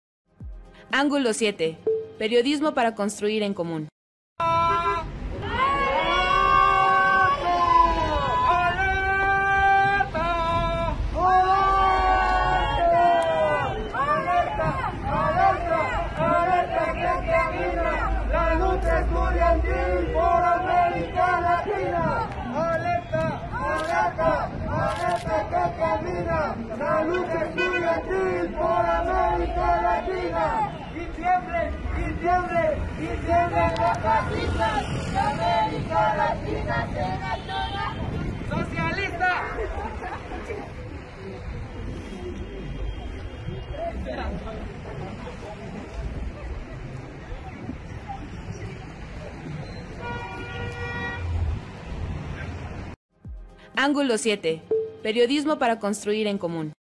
Frente a FGE, acusan nulo avance en agresiones a estudiantes de BUAP en 2015
Estudiantes del Colectivo Universitario para la Educación Popular (CUEP) se manifestaron frente a la Fiscalía General del Estado (FGE) para exigir justicia tras los actos de represión a alumnos de la BUAP en febrero de 2015, cuando mantenían una protesta pacífica.